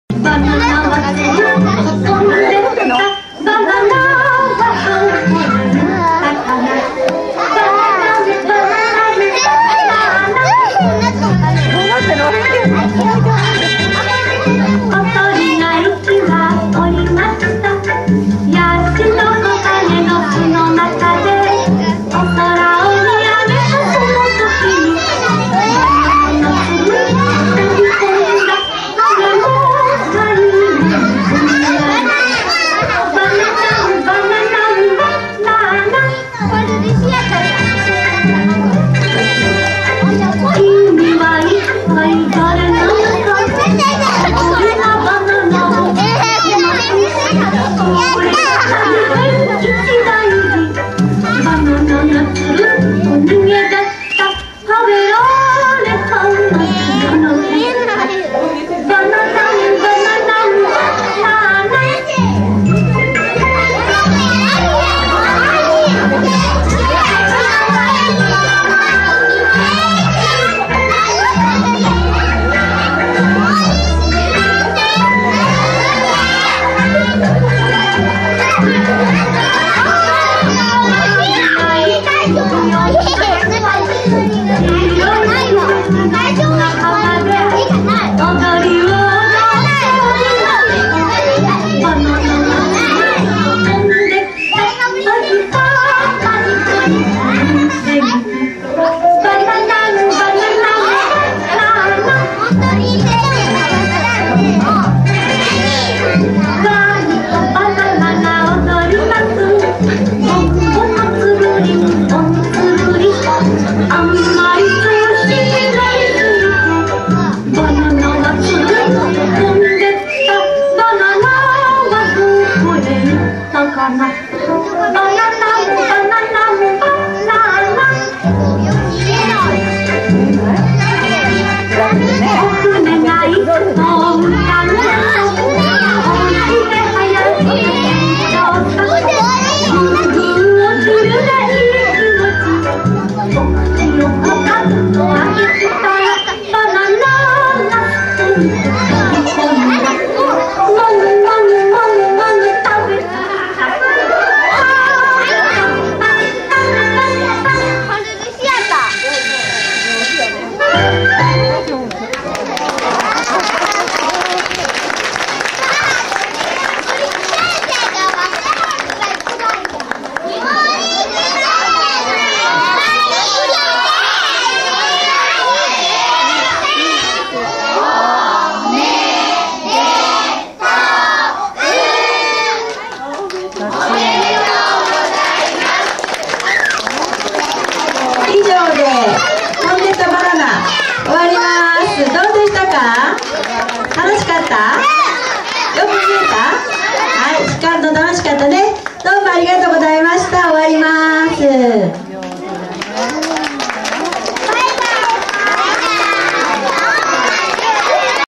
Sotsuensiki@Hoikuen
The Senseis of Goikuho Hoikuen performed during the Graduation Day post-party